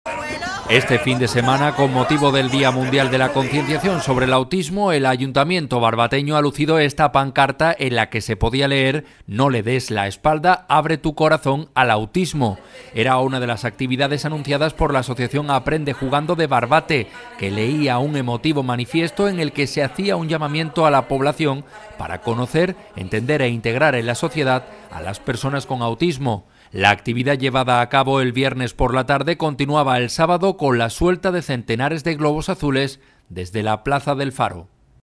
8NOTICIAS JANDA - 04ABR2016
La asociación barbateña "Aprende Jugando" nos hacía llegar las imágenes de la colocación de una pancarta reivindicativa en el Ayuntamiento de Barbate. Todo con motivo del día mundial sobre la concienciación del autismo.